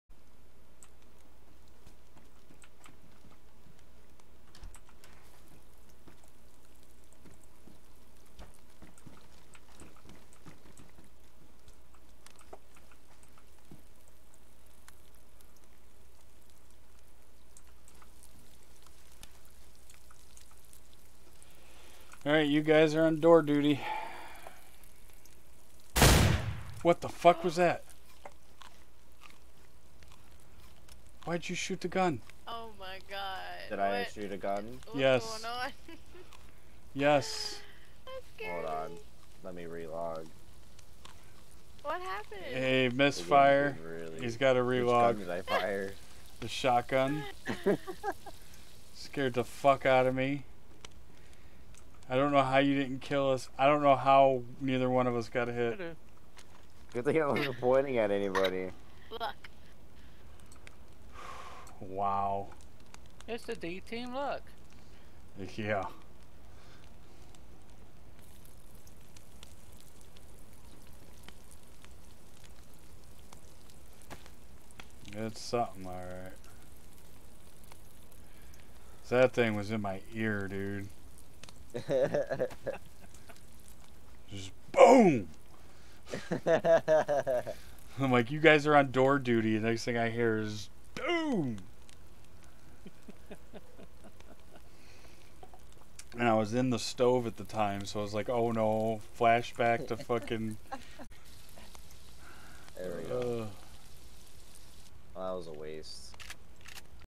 Again, GUN SAFETY people, learn it!!
Was trying to cook and about shite my pants!! And they want to arm the teachers lol -- Watch live